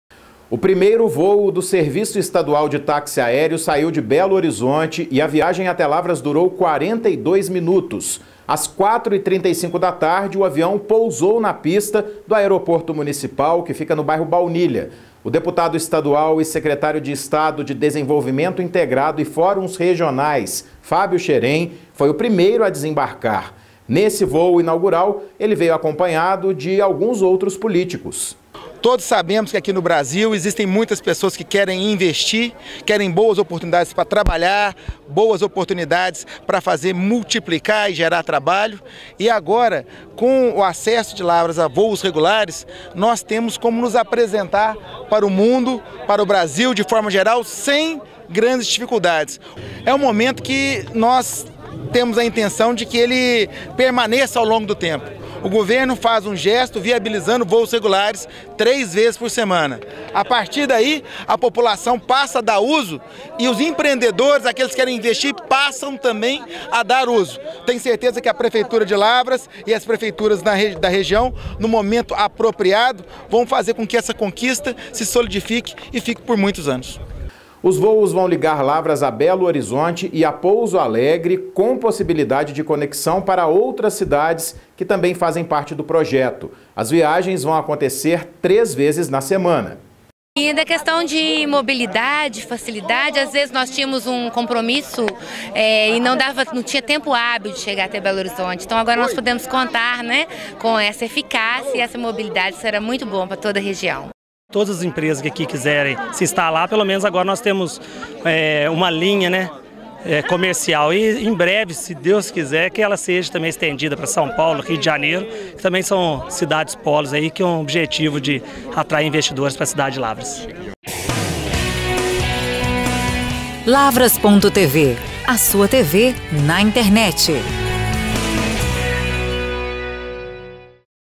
Primeiro Voo Comercial Lavras
Lavras foi incluída na segunda etapa do PIRMA, Projeto de Integração Regional de Minas Gerais no modo aéreo. Agora o município conta com três voos semanais para Belo Horizonte com possibilidade de conexão para outras cidades. Assista: